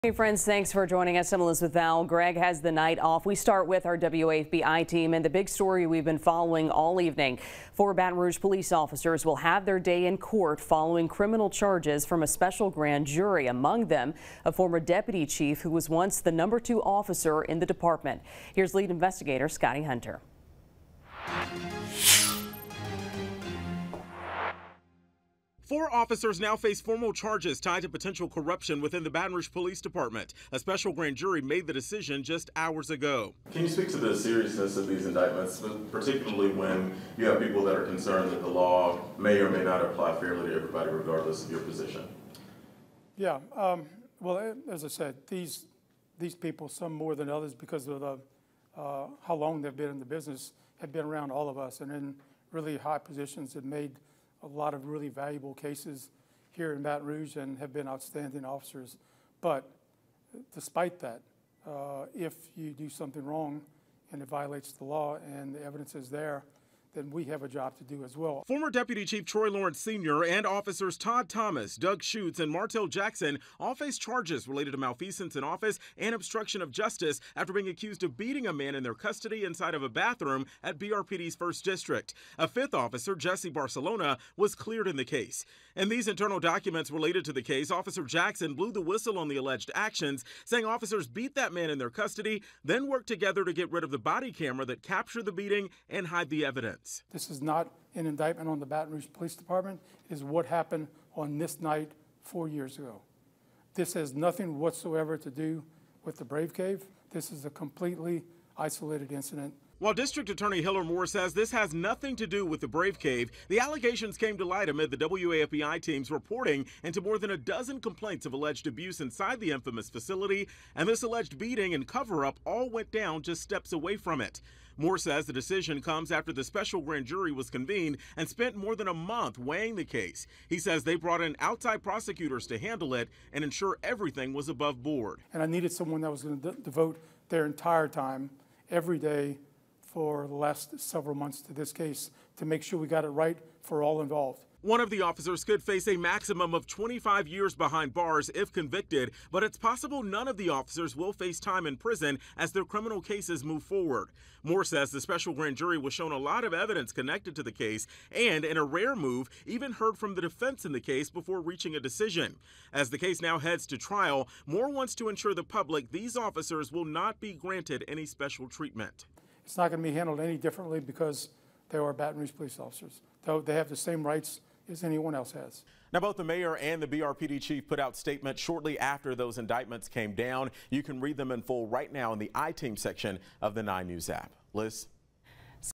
I-TEAM: DA weighs in following officer indictments
District Attorney Hillar Moore is weighing in, hours after a special grand jury decided four officers would face formal charges.